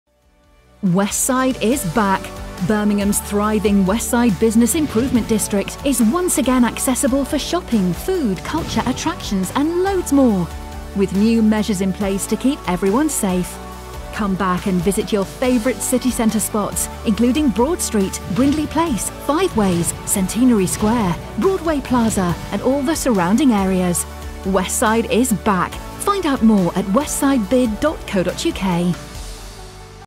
Welcome back to Westside!
Welcome back to the Westside district! With lockdown restrictions now being eased, we are pleased to invite you back to some of your favourite leisure and hospitality destinations.
to see the venues which are currently open and for regular updates.